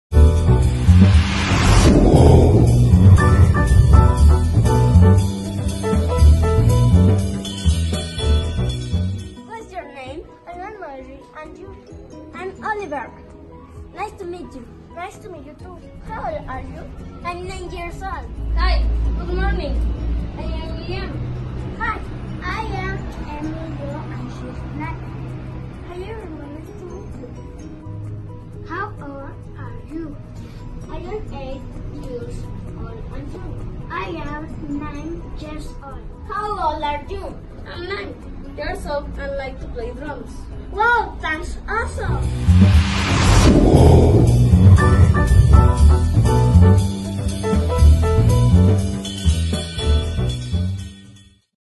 Axtli Simón Bolívar: "Greetings and Introductions"
Alumnos de 4° grado de primaria de la escuela Axtli Simón Bolívar en Banderilla Veracruz trabajando "Greetings and Introductions" de la clase de Inglés.